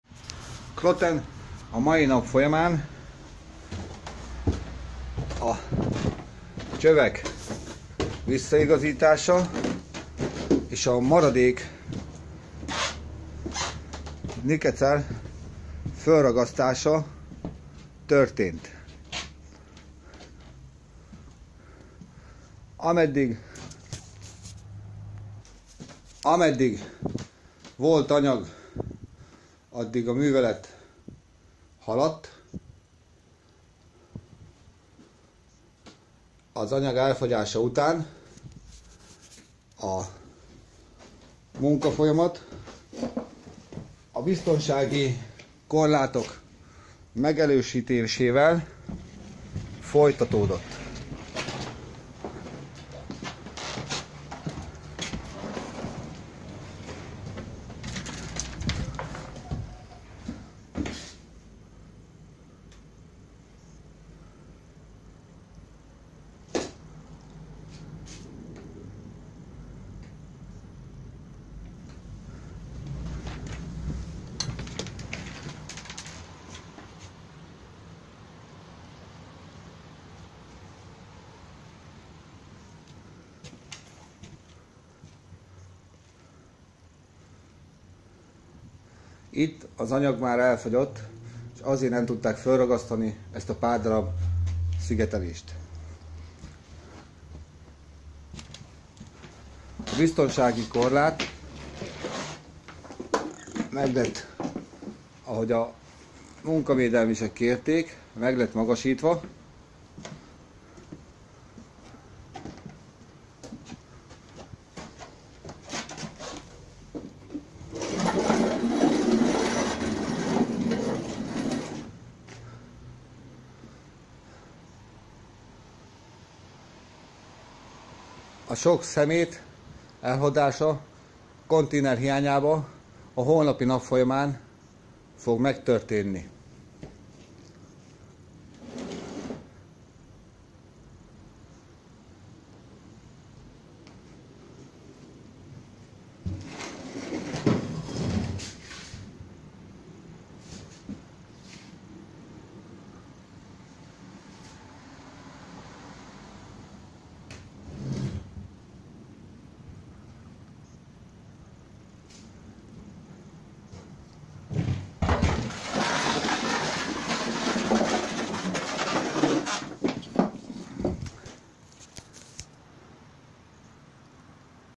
Kloten
Szigeteles es a vedö korlat felrakasa